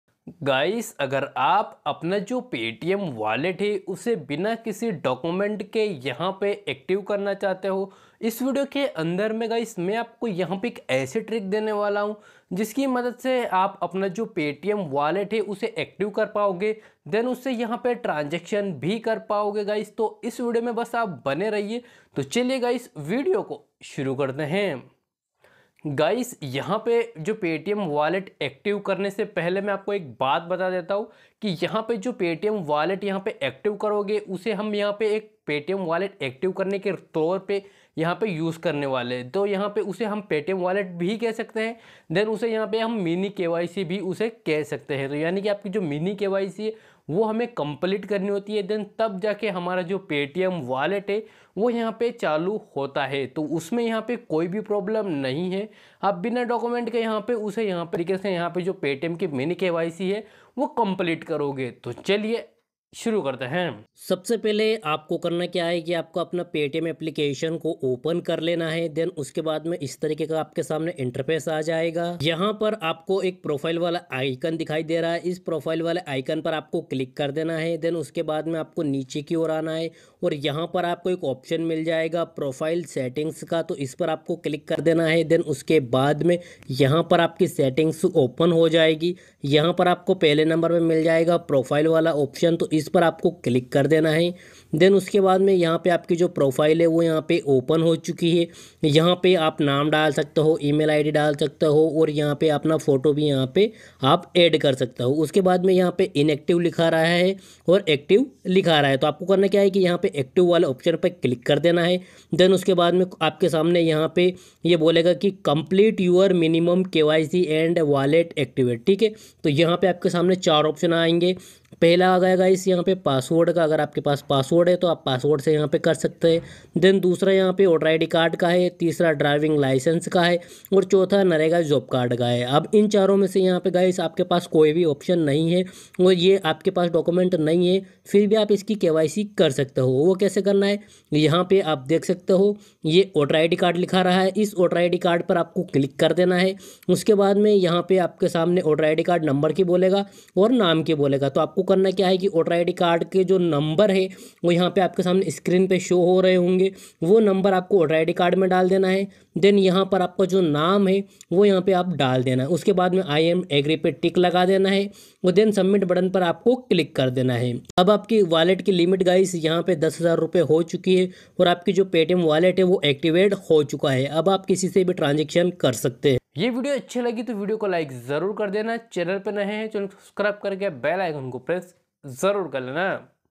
Paytm wallet activate kaise karen 2022 | how to paytm wallet activate
Doston is video mein main aapko paytm wallet activate Kar ke dikhaunga vah Bhi without kyc paytm wallet activate 2020 ios hindi paytm wallet activate

Best Gadgets & Youtube Equipements I had used & recomend to Youtubers :

1. Mobile Use
2. Boya M1 Microphone
3. Green Screen
4. Black Screen
5. TRIPOD FOR MOBILE